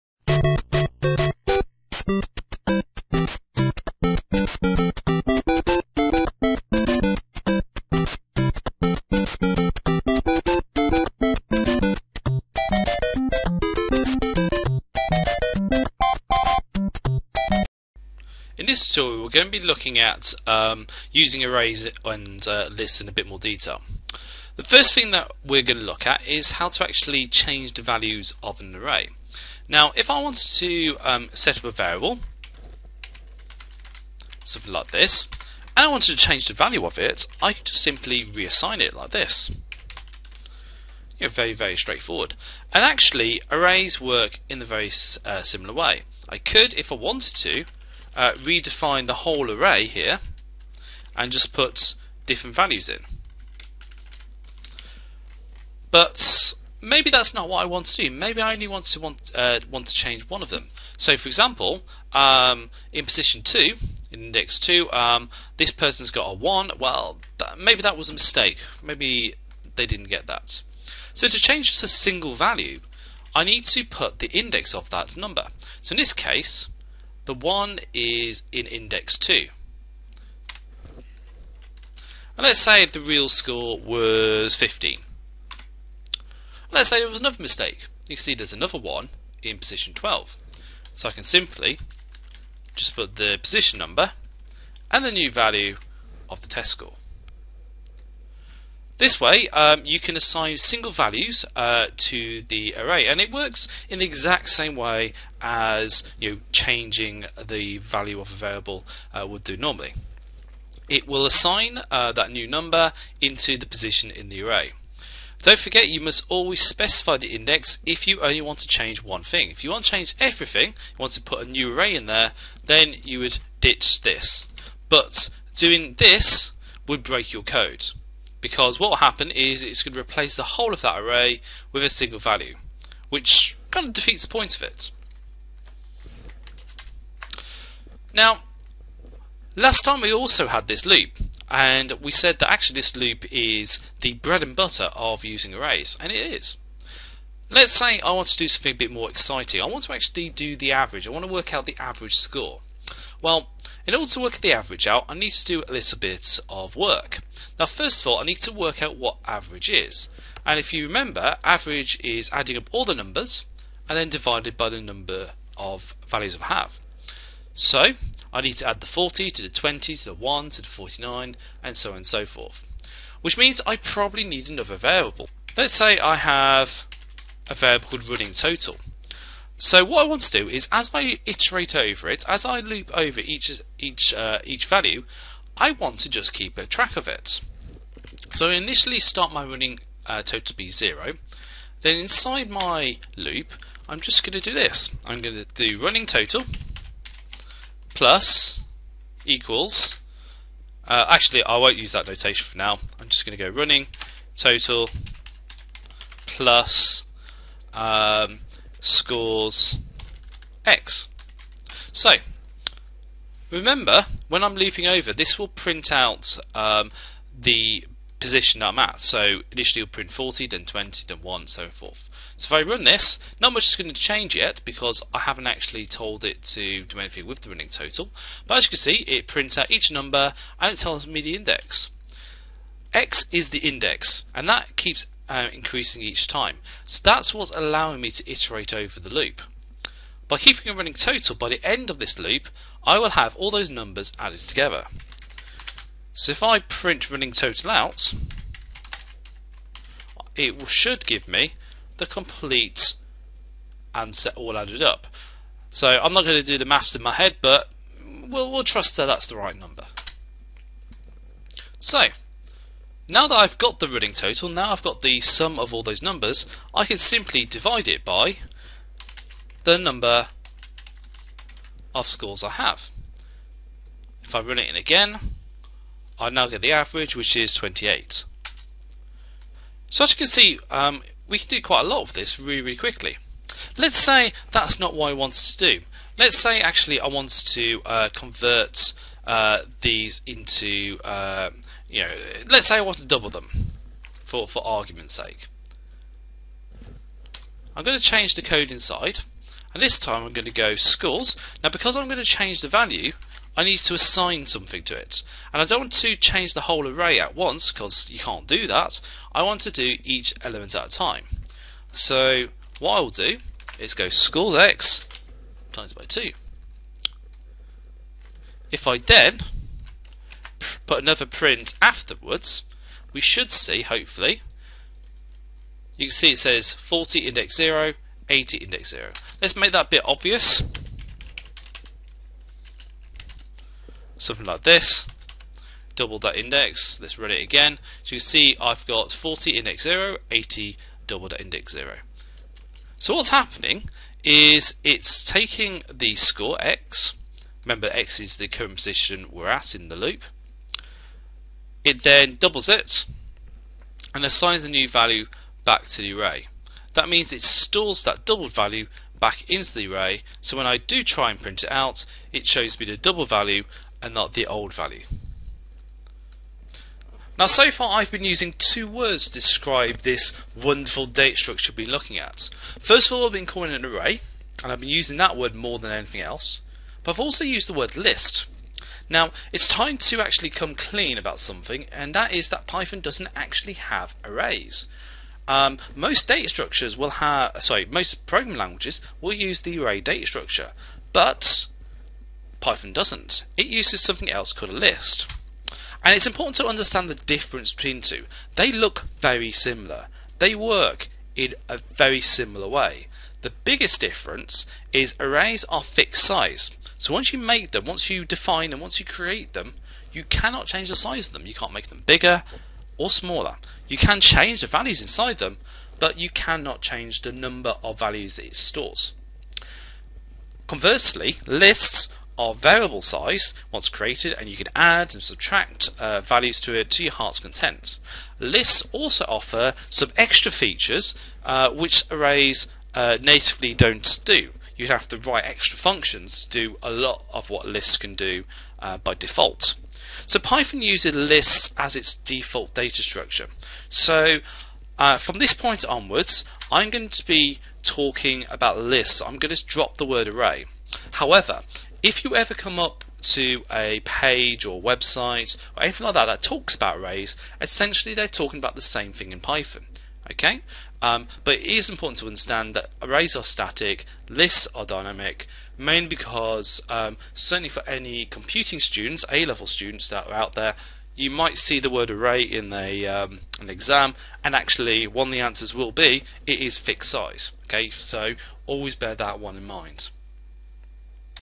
Using python lists part 2
This video will show how to assign values to an array and will try and explain the key difference between an array and a list-